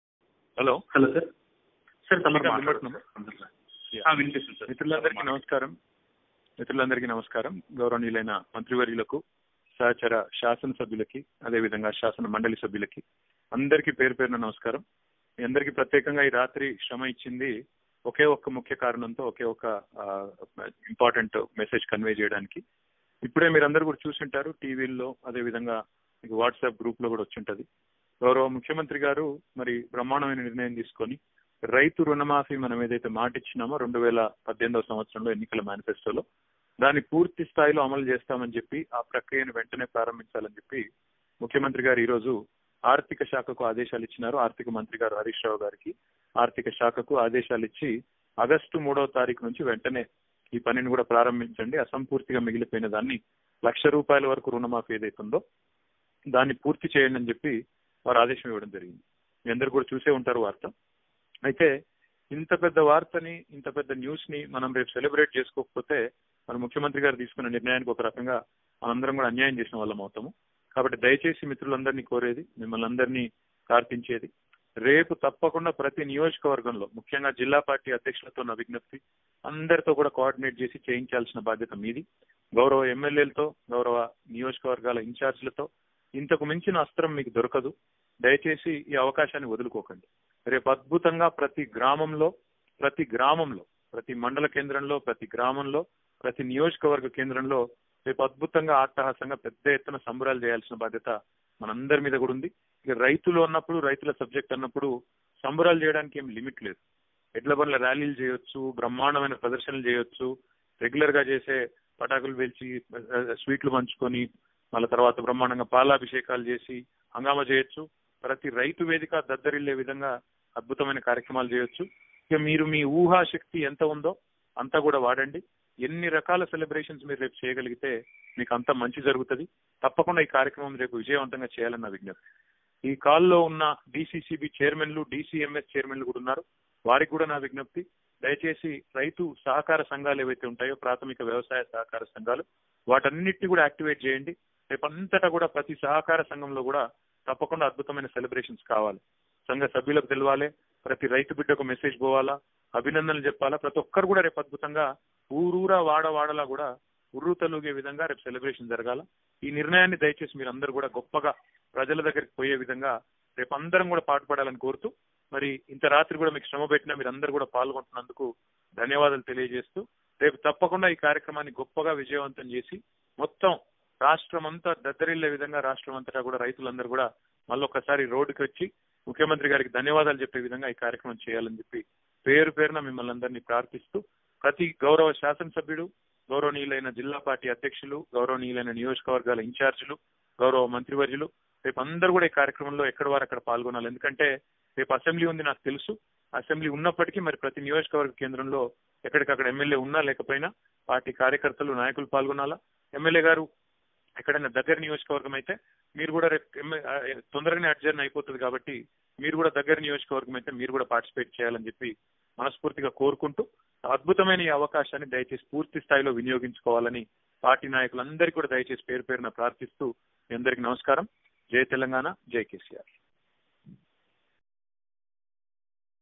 పాలాభిషేకాలు చేయండి ...!!అద్భుత అవకాశం వదులుకోకండి !కెటిఆర్ ఆడియో డ్రామా !| KTR Call Audio leaked
పాలాభిషేకాలు చేయండి ...!!అద్భుత అవకాశం వదులుకోకండి !కెటిఆర్ ఆడియో డ్రామా !| KTR Audio leaked